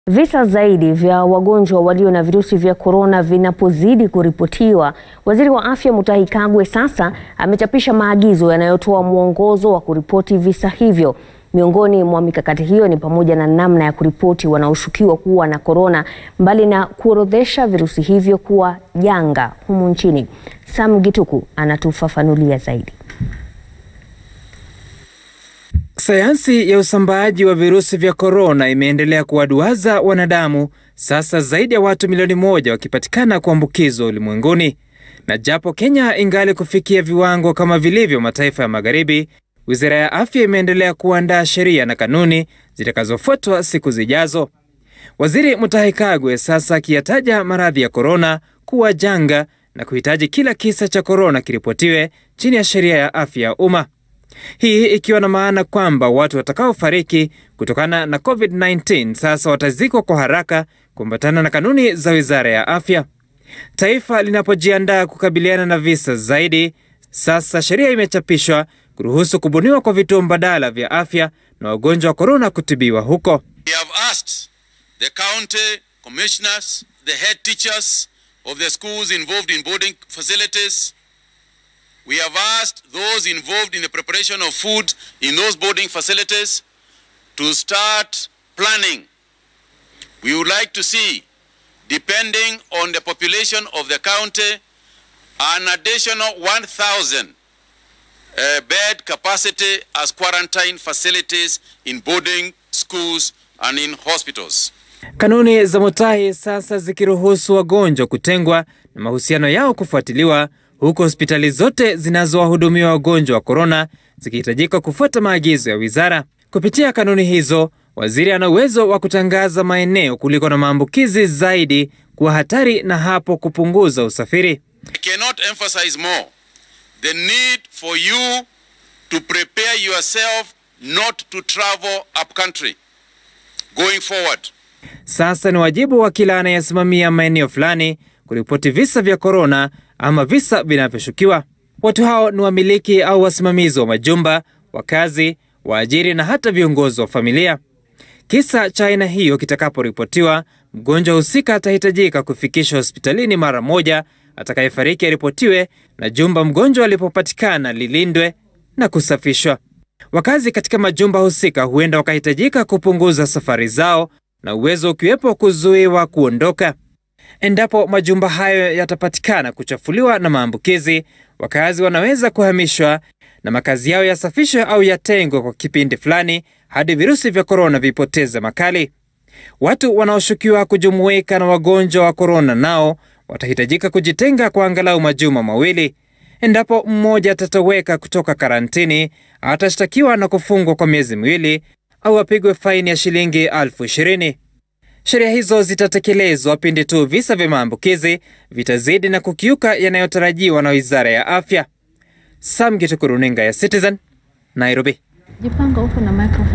Maagizo ya Corona yachapishwa katika gazeti rasmi la serikali
Visa zaidi vya wagonjwa walio na virusi vya corona vinapozidi kuripotiwa, waziri wa afya Mutahi Kagwe sasa amechapisha maagizo yanayotoa muongozo wa kuripoti visa hivyo. Miongoni mwa mikakati hiyo ni pamoja na namna ya kuripoti wanaoshukiwa kuwa na corona mbali na kuorodhesha virusi hivyo kuwa janga nchini.